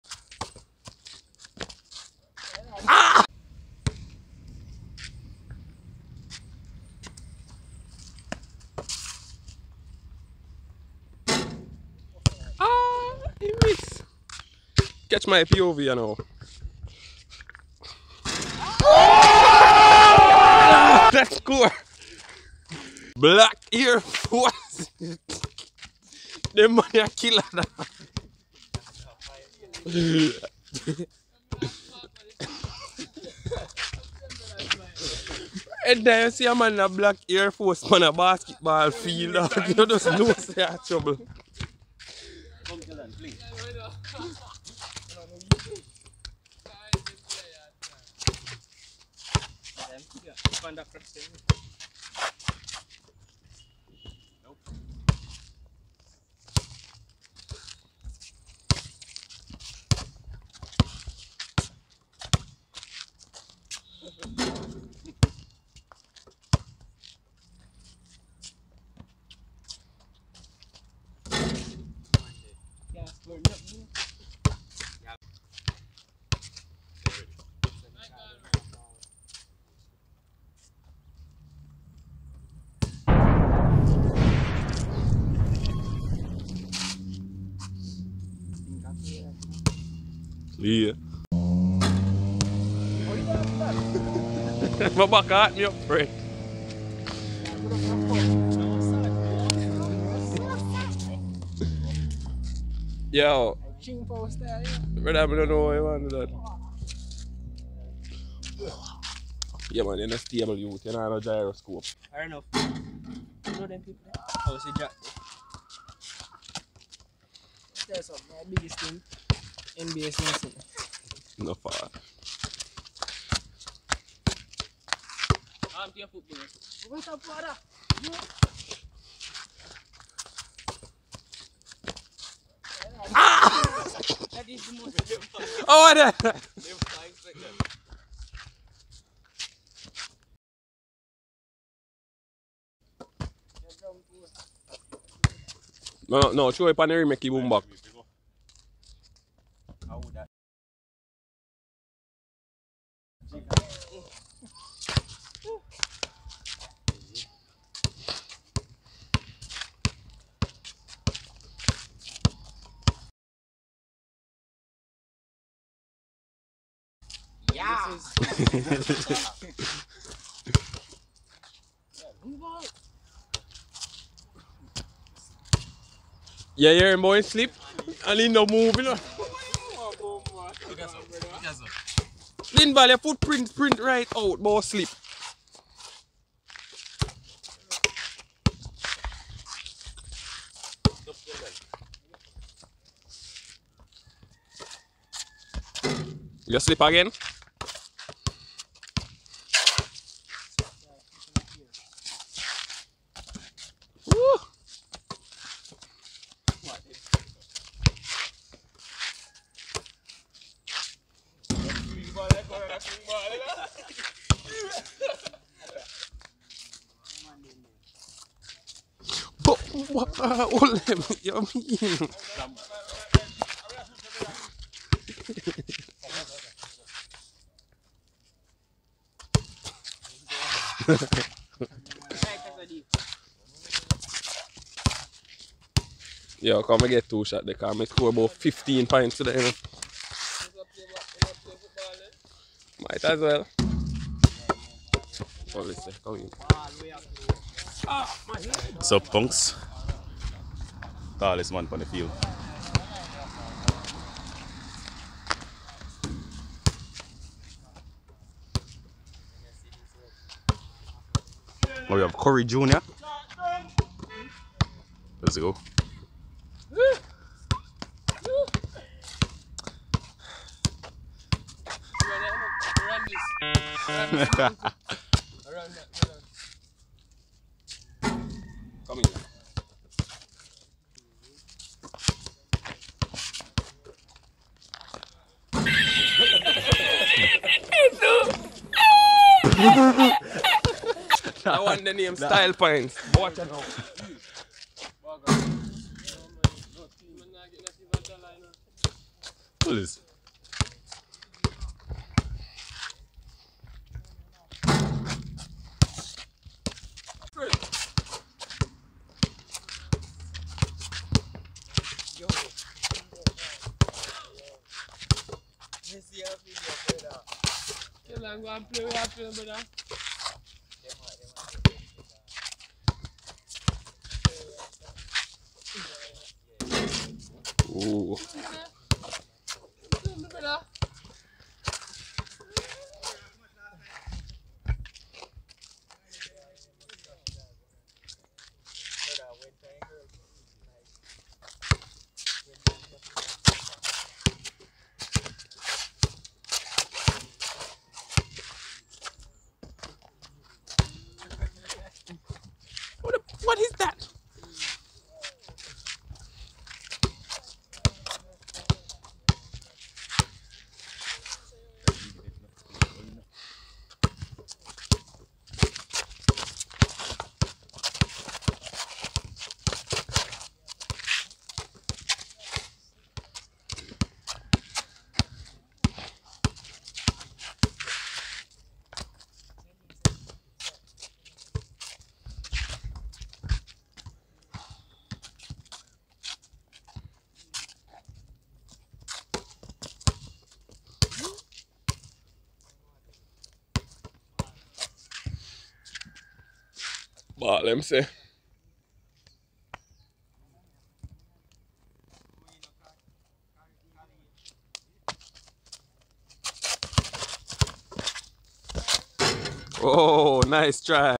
Black Air Force Energy || We cant shoot or dunk // behind the scenes funny moments 😂😂
Today's video is about how our mornings are like before we play Basketball in Jamaica alot of funny moments
NBA in Jamaica